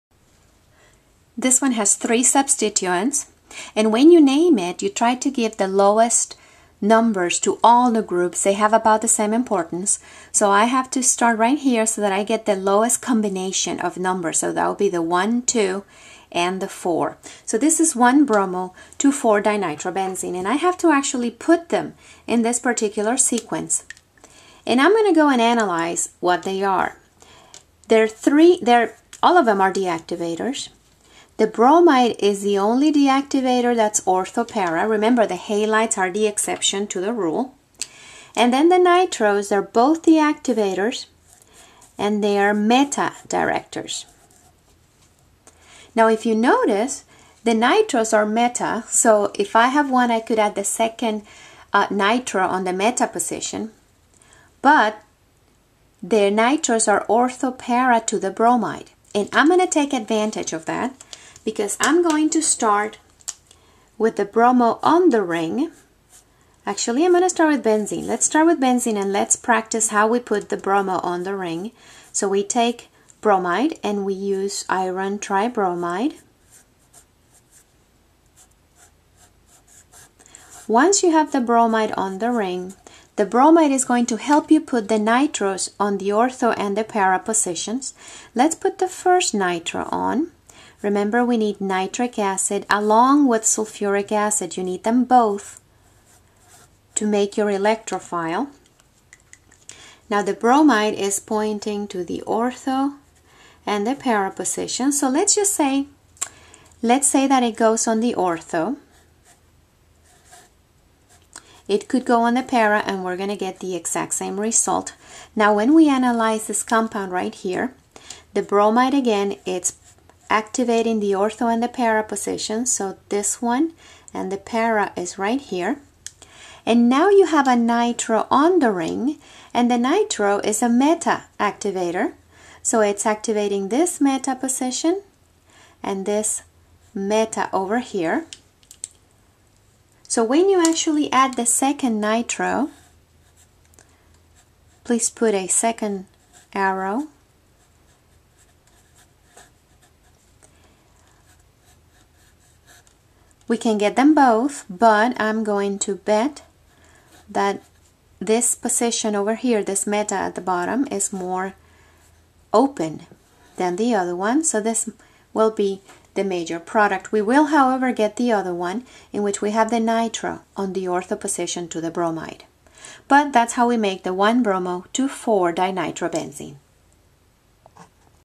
EAS; Problem 3 1-br-24-dinitro.mp4
How would you prepare 1-bromo-2,4-dinitrobenzene?